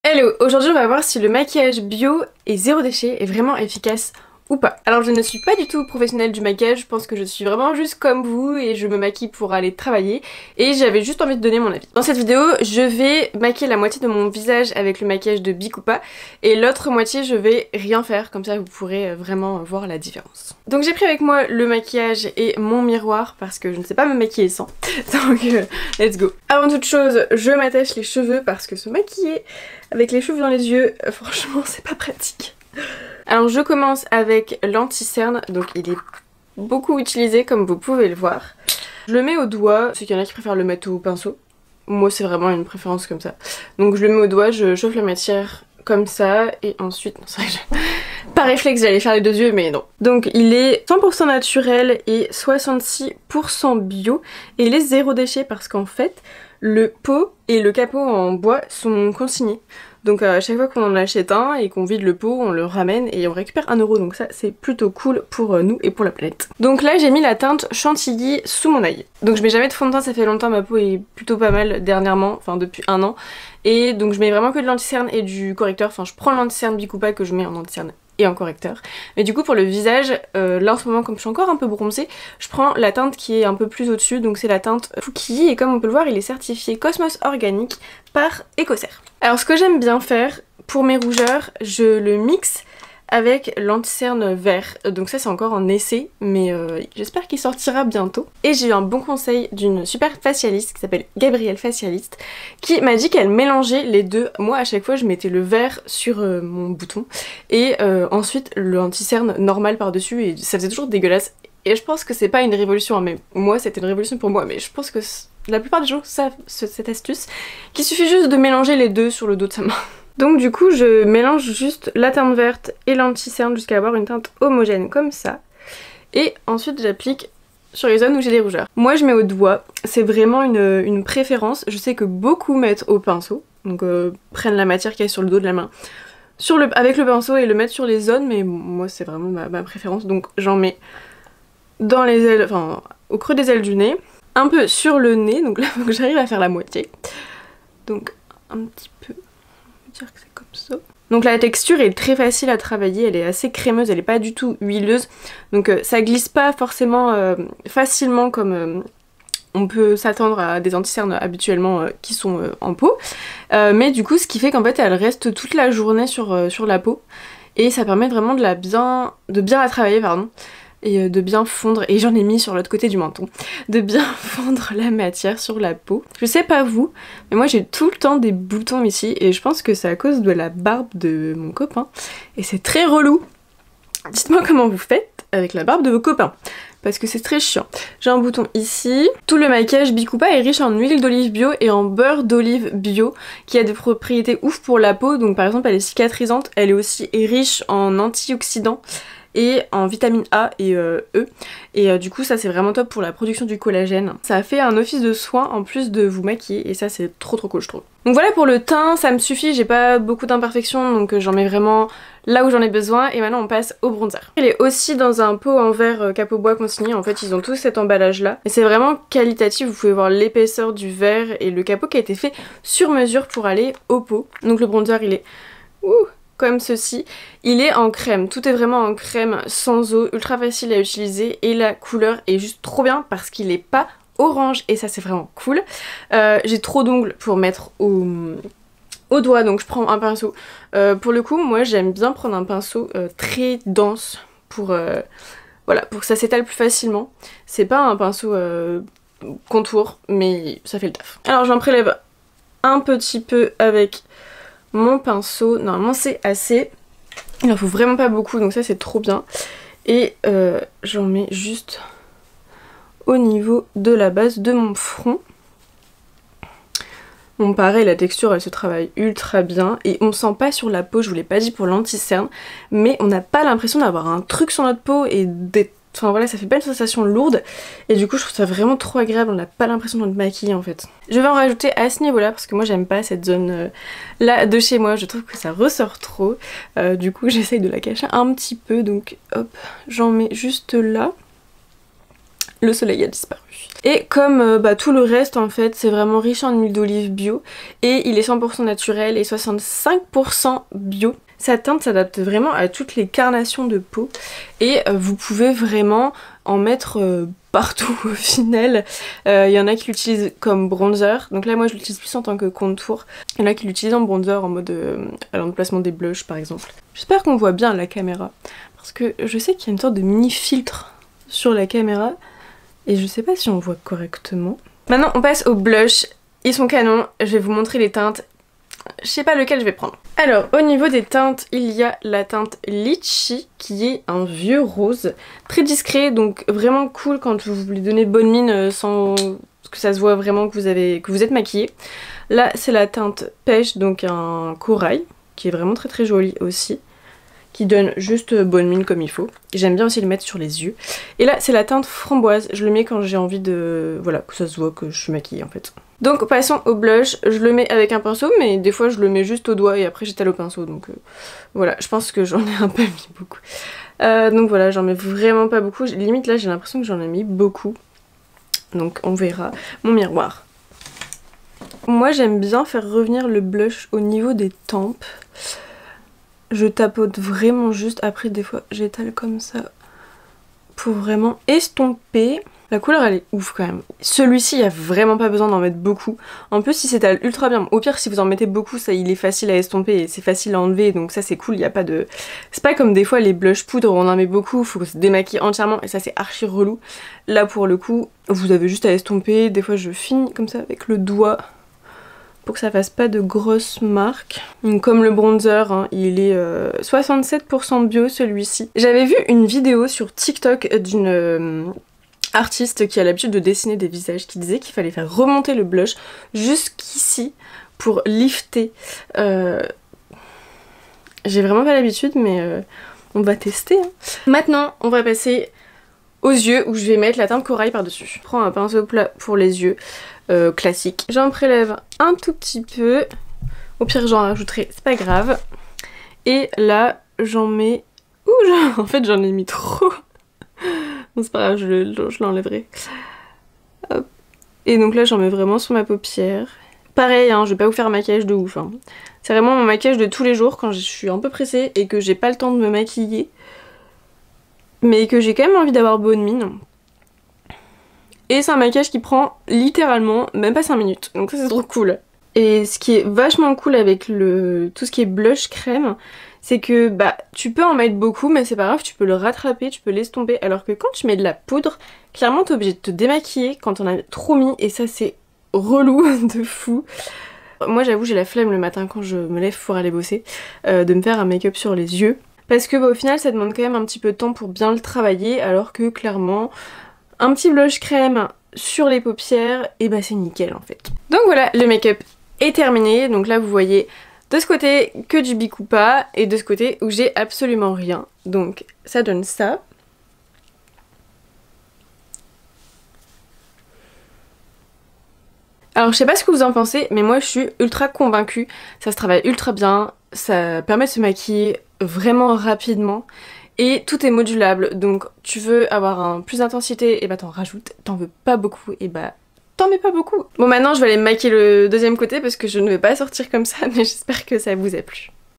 Je teste du maquillage bio sur la moitié de mon visage !
❤ PLUS D'INFOS JUSTE ICI :

Je teste du maquillage bio sur la moitié de mon visage, le maquillage BIKUPA qui est zéro déchet.